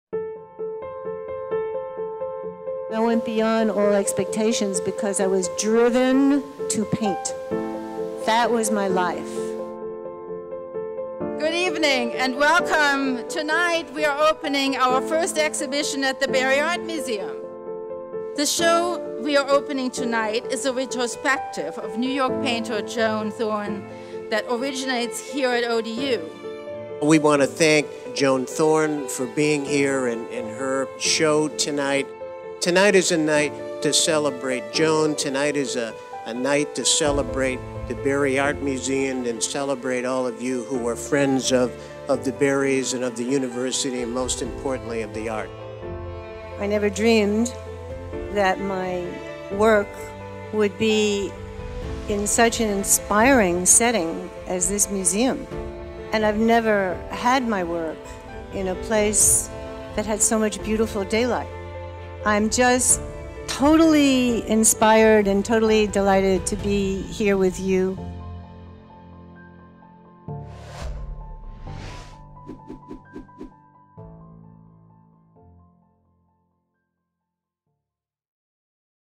Joan Thorne Exhibit Opening at the Barry Art Museum
The first temporary exhibition at Old Dominion University's Barry Art Museum features the complex, chromatic work of American abstract painter Joan Thorne. This retrospective, "Joan Thorne - Light, Layers, Insight," consists of 30 large-scale works (28 oil paintings on canvas and 2 drawings) spanning the artist's career from the early 1970s to 2018.

Large in scale, the strong visual effects in Thorne's paintings simultaneously focus and dissolve when seen up close. A challenge to be captured digitally and in print, they invite contemplation and demand to be seen in person.

The exhibition is accompanied by a fully illustrated exhibition catalog published by the museum with essays contributed by Vittorio Colaizzi, associate professor of art at Old Dominion University, and Richard Vine, managing editor of "Art in America" magazine that place Thorne's artistic works in the broader context of modern and contemporary art.

The exhibition is free and open to the public and runs through May 10, 2020. The museum is located at 1075 W. 43rd St., Norfolk, Va.

FOLLOW ODU on social media!